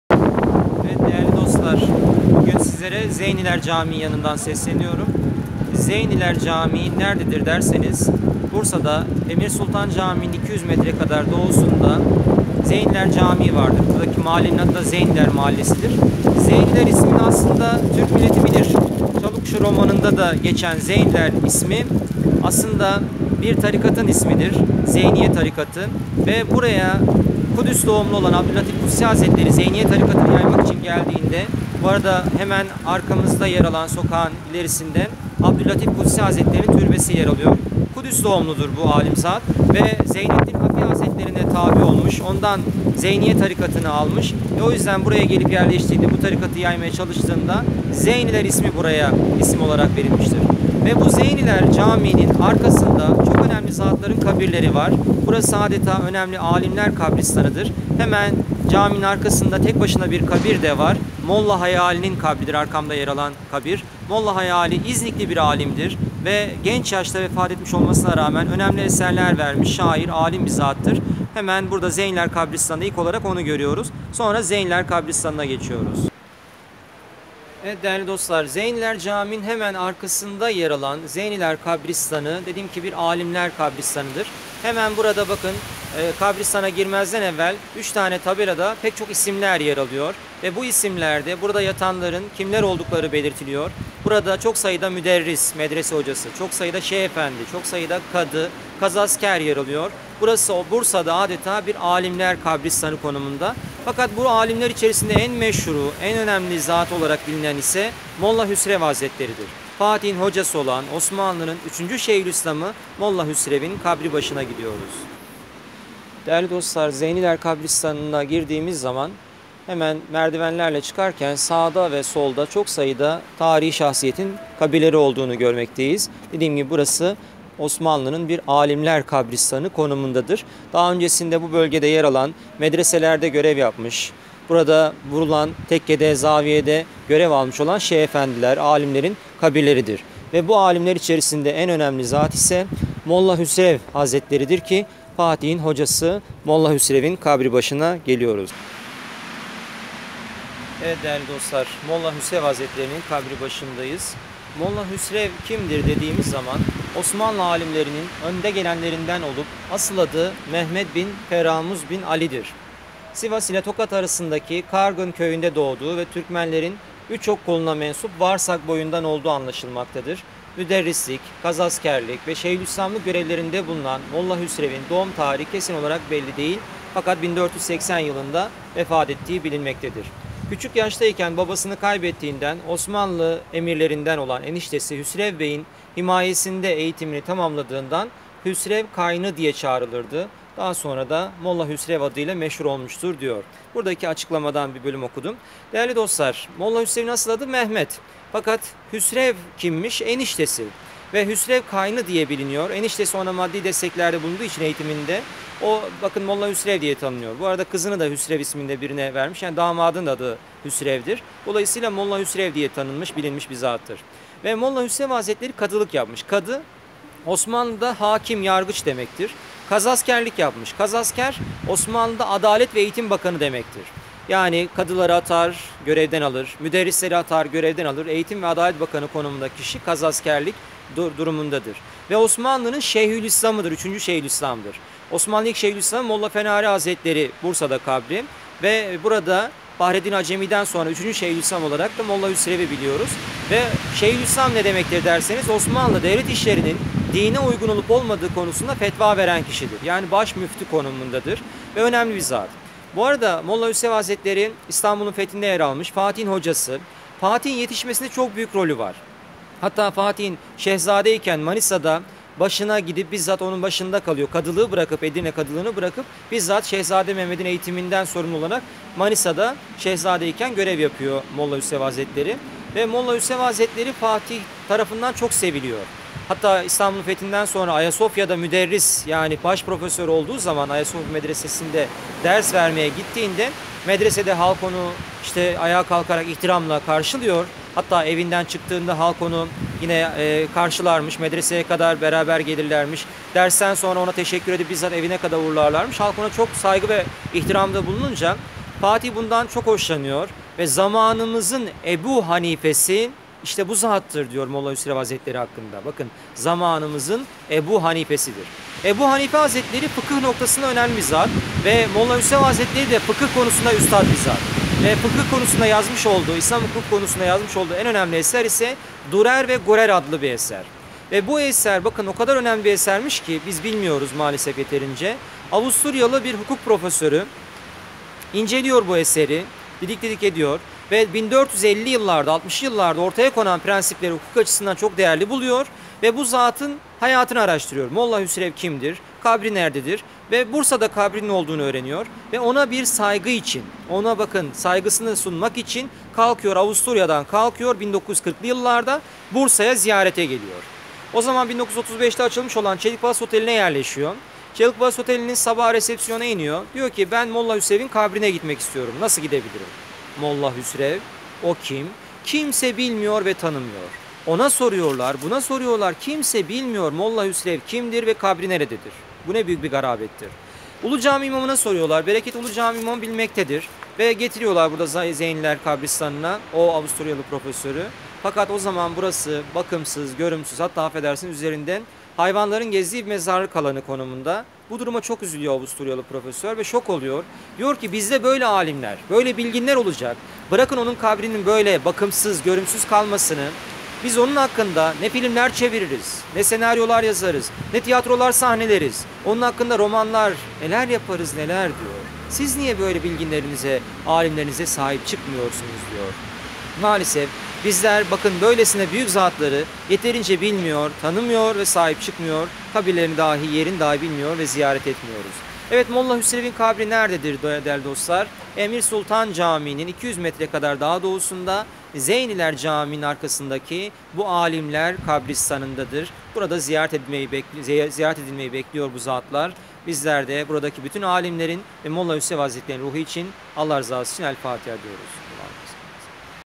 Fatih'in Hocası Molla Hüsrev - Zeyniler (Bursa)
Bursa'da az bilinen Zeyniler kabristanı bir alimler kabristanıdır. Burada Fatih'in Hocası Molla Hüsrev'in kabri de yer alır.